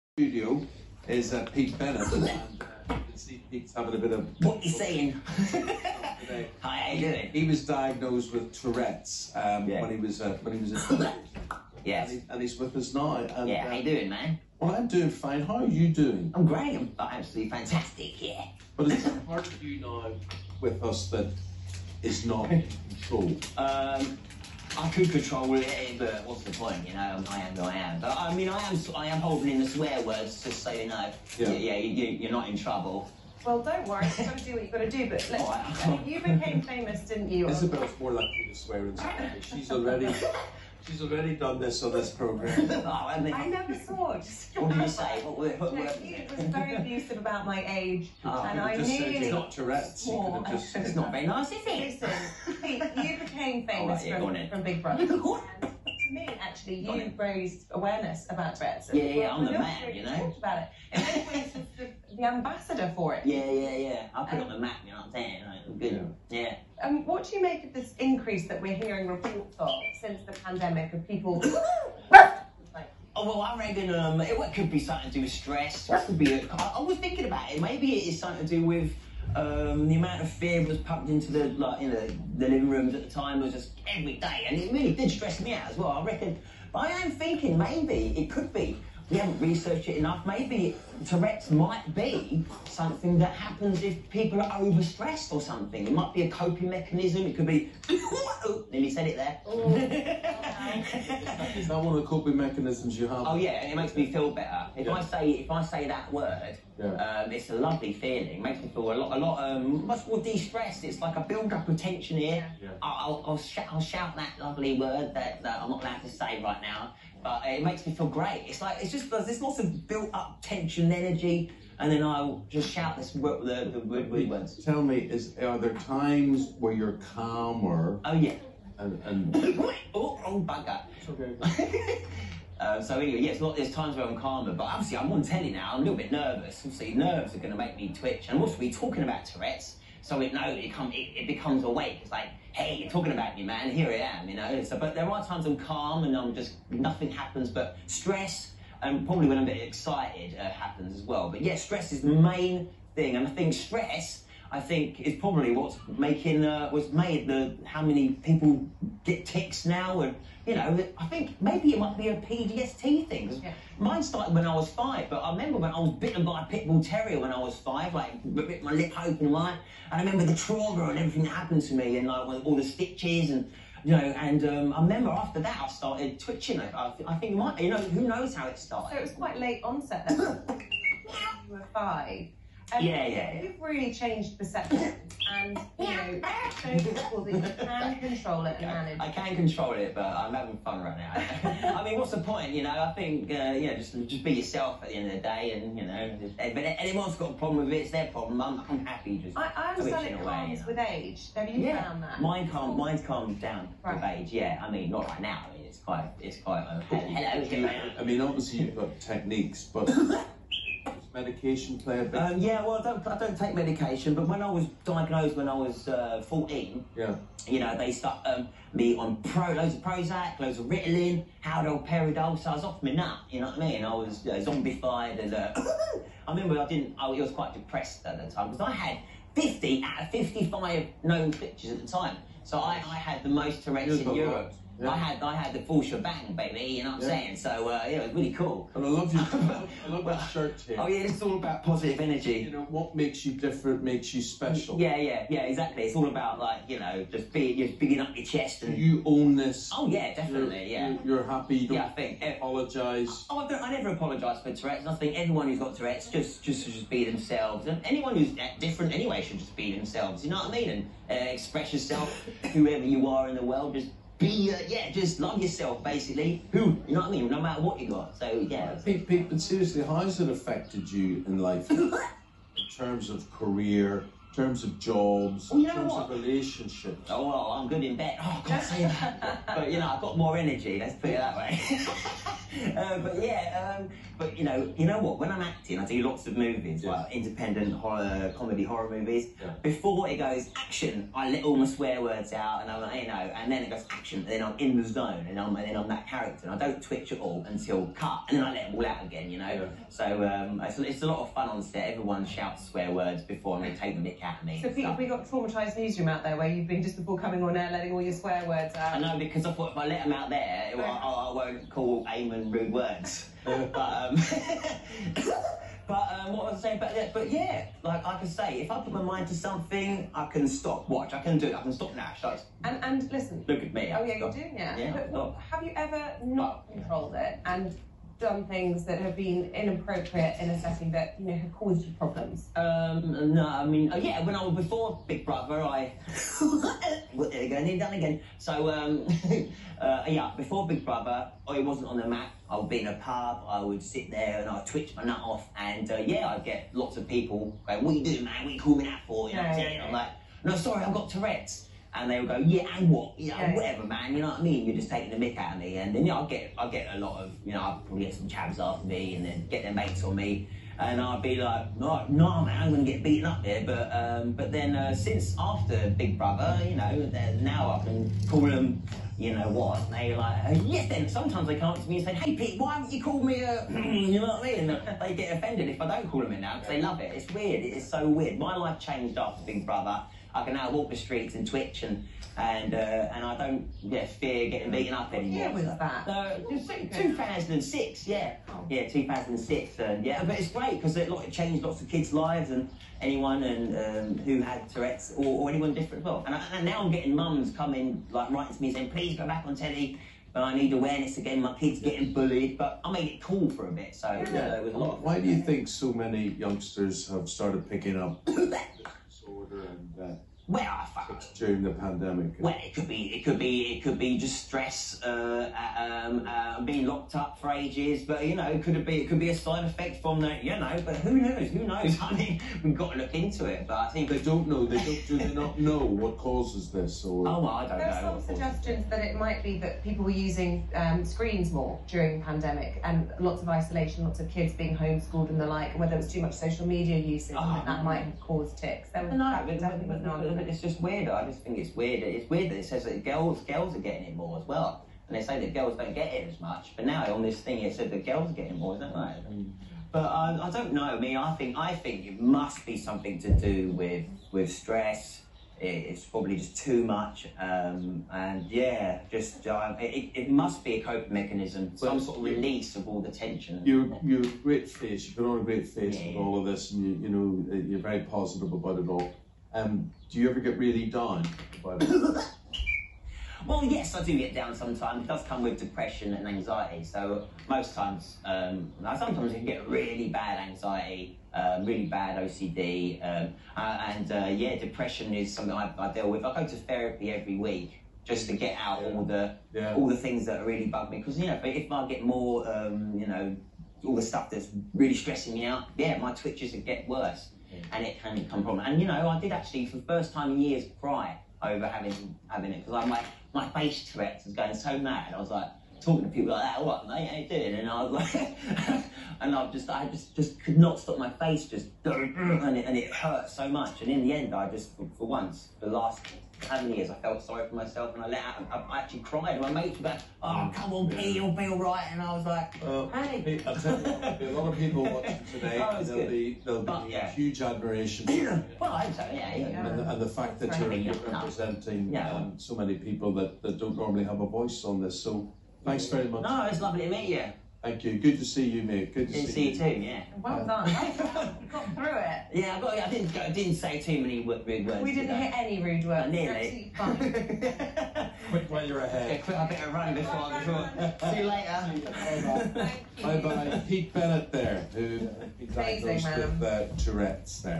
Pete Bennett on GB news TOURETTES
having a chat with eamon holmes